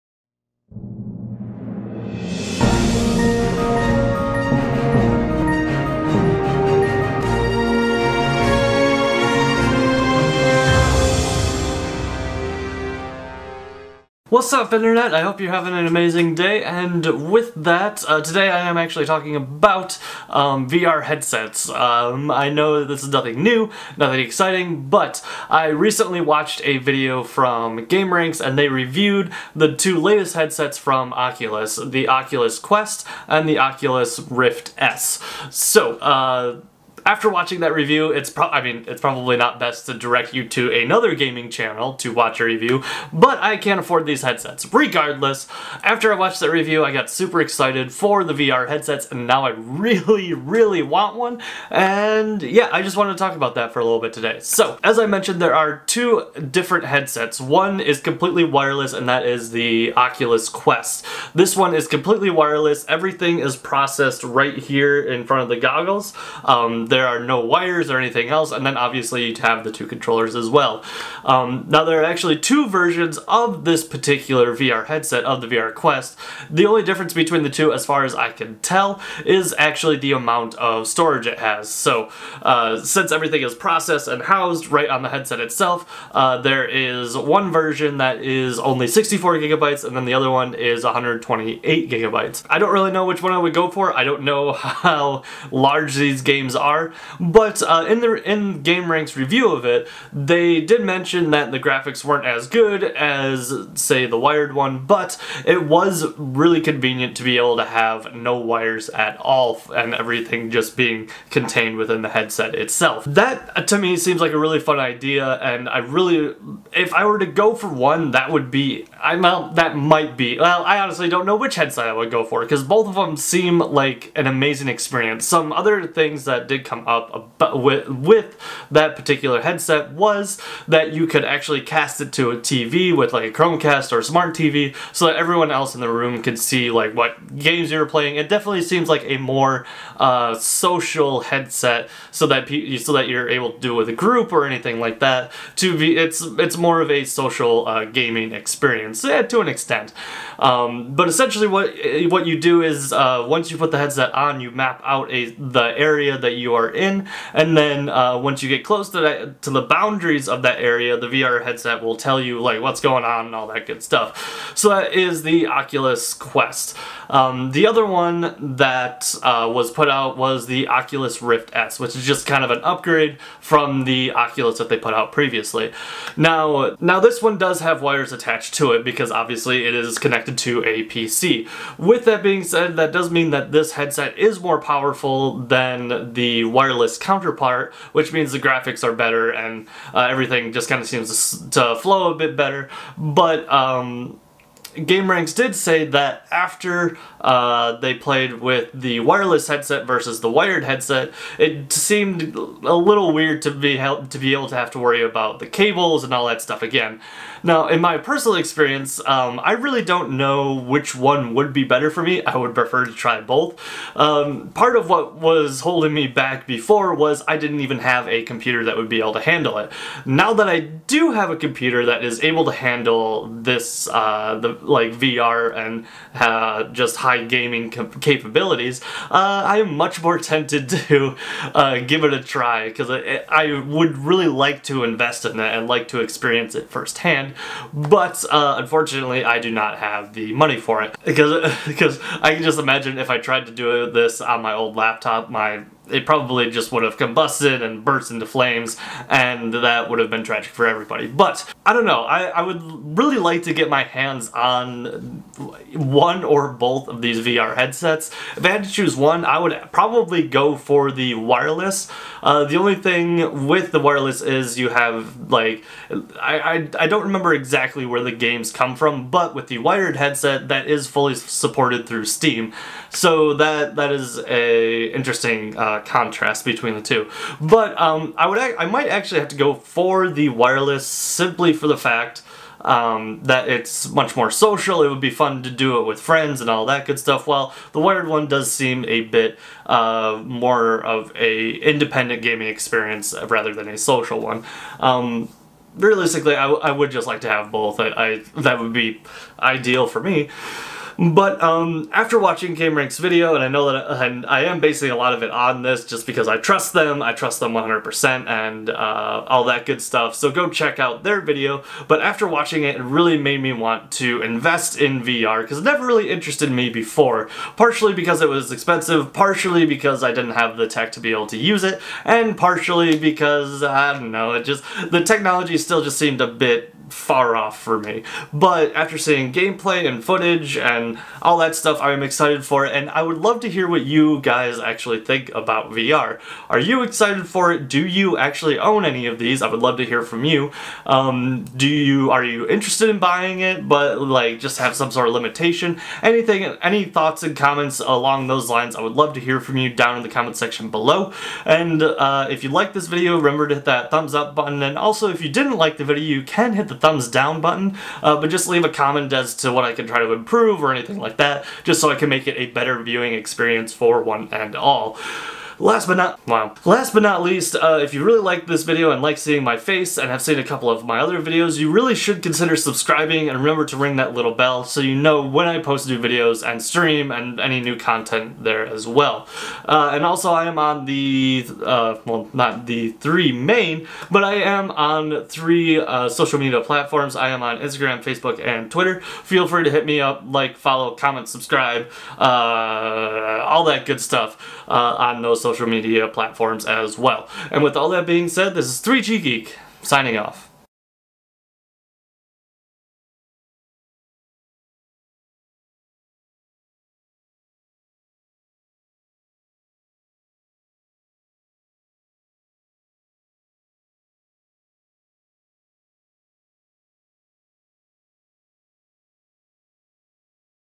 VR Excitement!!!!
Hello one and all! Today I just wanted to talk about VR and the review that I recently saw on Gameranx. I never really had an interest in VR until after I saw their review on the two releases from Oculus. Now I have the extreme want for one but preferably both of these VR headsets!